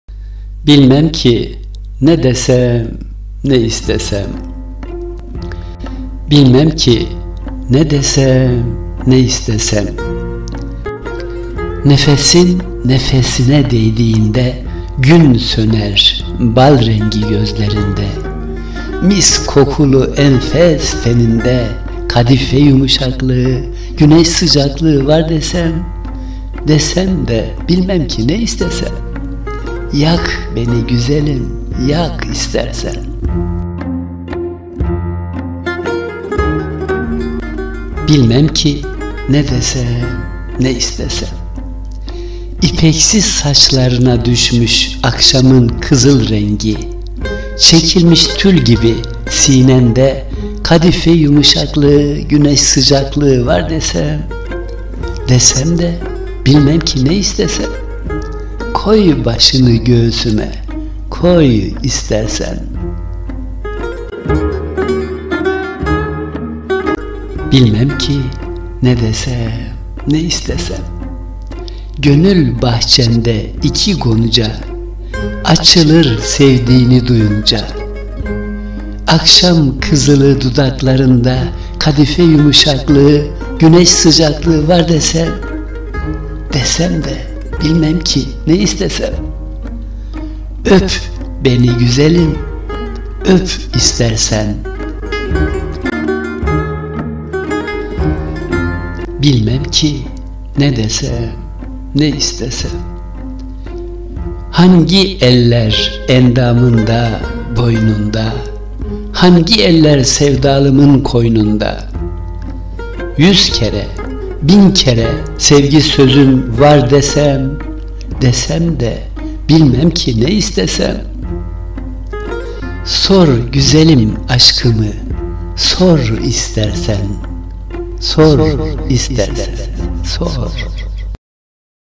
Bilmem ki Ne Desem Ne İstesem - Hikmet Çiftçi
Bilmem ki
   Ne desem
      Ne istesem?
‘Nefesim nefesine değdiğinde
 Gün söner bal rengi gözlerinde’

Mis kokulu enfes teninde
Kadife yumuşaklığı
Güneş sıcaklığı
Var desem
   Desem de
       Bilmem ki, ne istesem?

Yak beni güzelim
Yak istersen...
Hikmet Çiftçi
NOT: ÖNCEKİ VİDEO FON MÜZİK OLMASINA RAĞMEN "MÜZİK TE'LİFİ" NEDENİYLE KALDIRILDI.